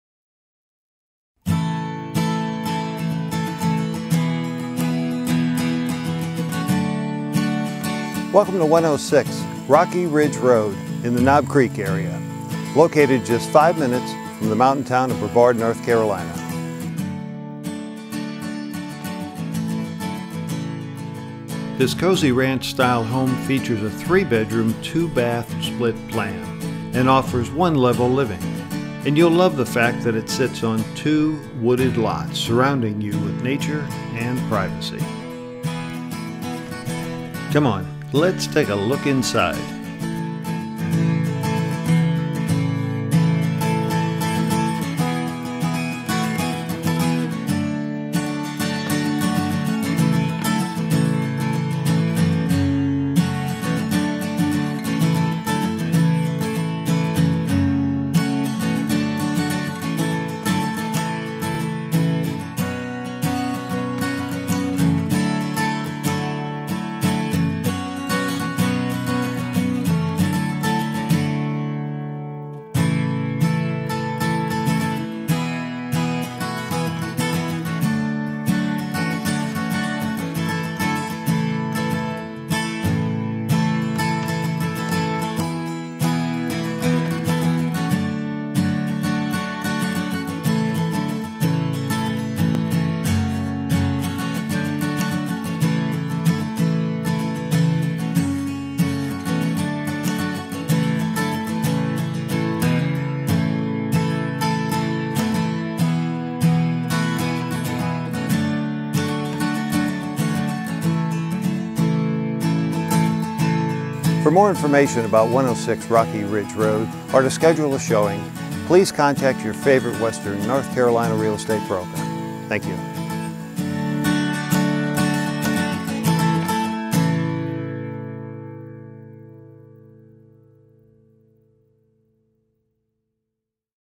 Sold by The Clay Team - 106 Rocky Ridge Road, Pisgah Forest NC
One level living, a private double lot, close to town! This is a cozy 3 br/2 bth ranch with a split plan, large master bedroom and eat-in kitchen. The home is nestled in a private wooded setting that is great for nature lovers, bird watchers, and anyone looking for peace and quiet. The adjacent .46 ac lot is included and offers added privacy or possible add'l building site. The unfinished basement has high ceilings and outside entrance and has great potential as a workshop.